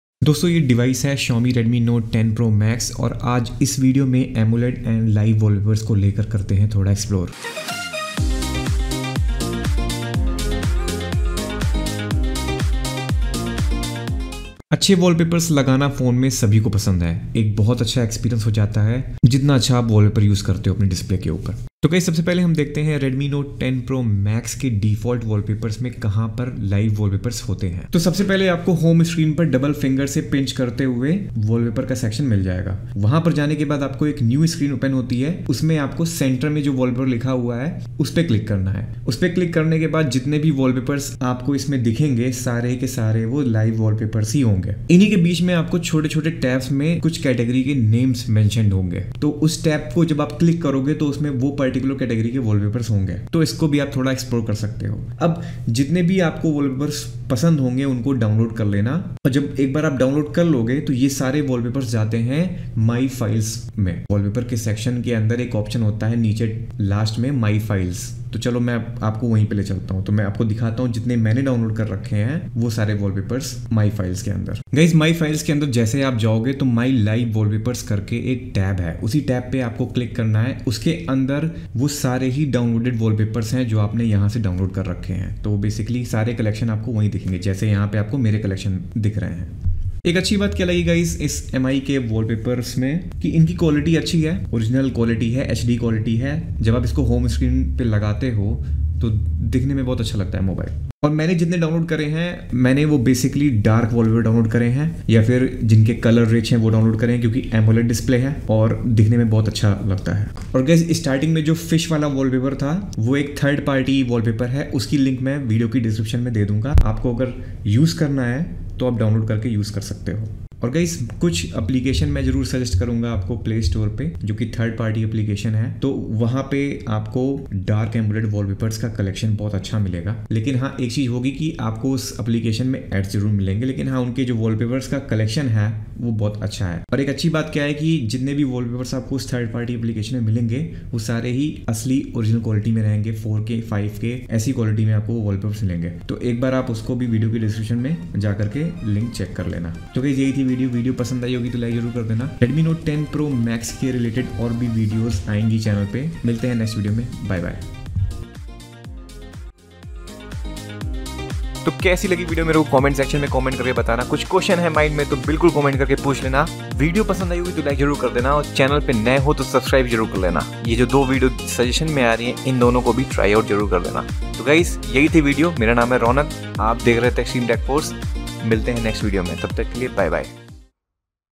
Redmi Note 10 Pro Max | Live Wallpaper Setup
Hey Everyone!
Dosto Aaj ki video me maine Xiaomi ke Live & Dark Amoled wallpapers ke baare me bataya hai toh video ko pura end tak zarur dekhe

Wallpaper Link Coming Soon

Do checkout our official facebook page-